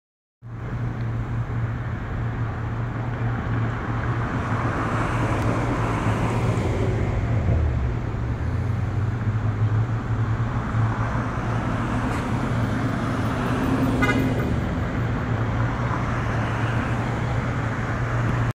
South Van Ness Ave/20th St San Francisco
Red light runner 3/8/16..SFMTA doesn't care.